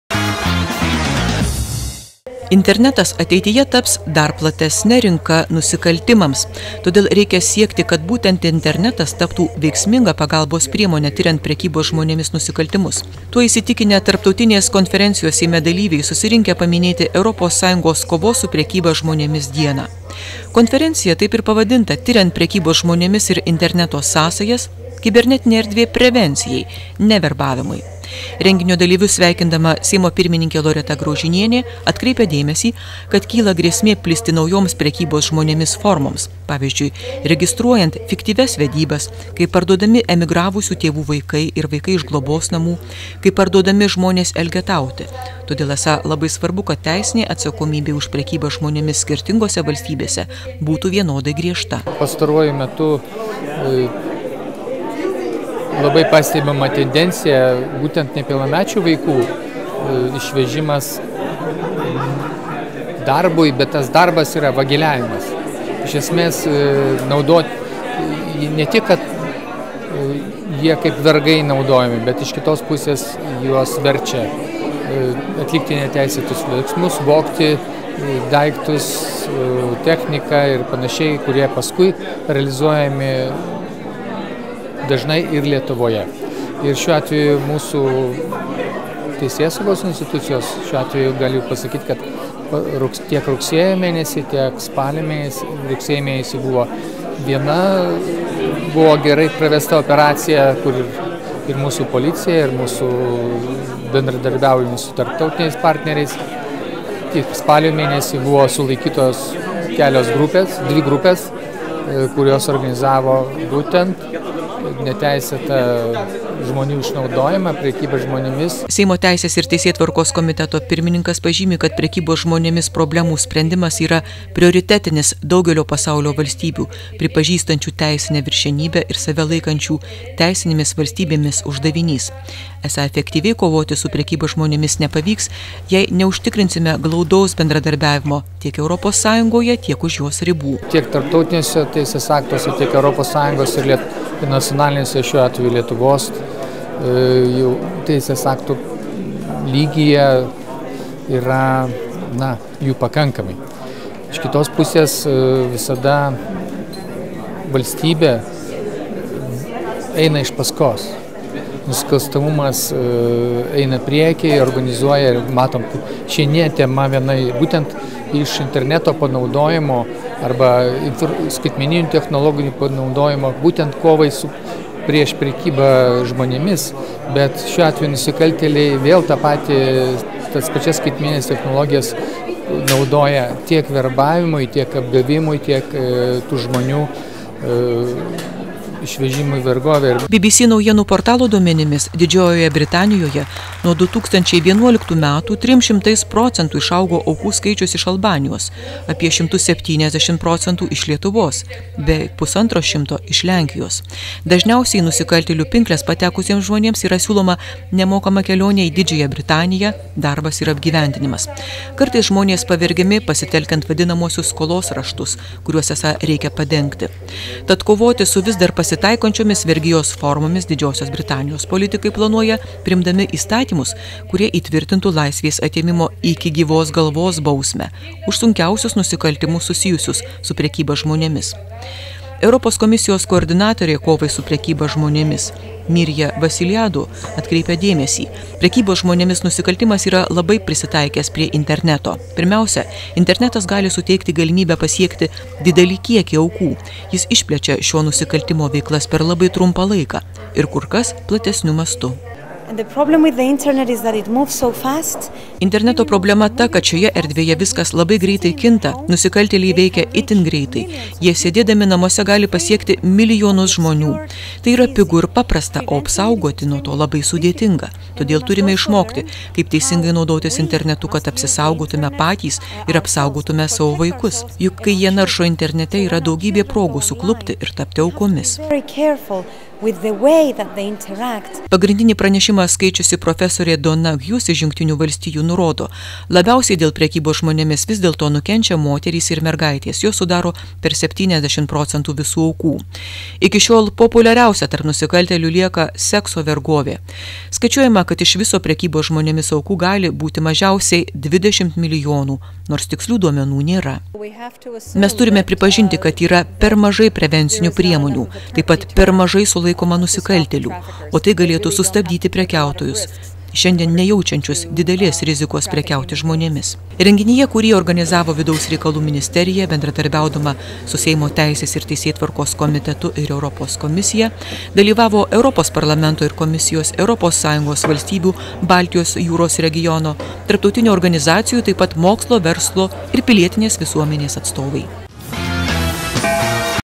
Tarptautinė konferencija Seime, skirta ES kovos su prekyba žmonėmis dienai paminėti
2013 m. spalio 18 d. įvykis. Tarptautinės konferencijos Seime dalyviai teigia, kad internetas ateityje taps dar platesne rinka nusikaltimams, todėl reikia siekti, kad būtent   internetas  taptų veiksminga pagalbos priemone tiriant prekybos žmonėmis nusikaltimus.
Konferencija pavadinta „Tiriant prekybos žmonėmis ir interneto sąsajas: kibernetinė erdvė prevencijai -- ne verbavimui".
Redaktorė Saulė Gedrimaitė
Operatorius  Ričardas Davainis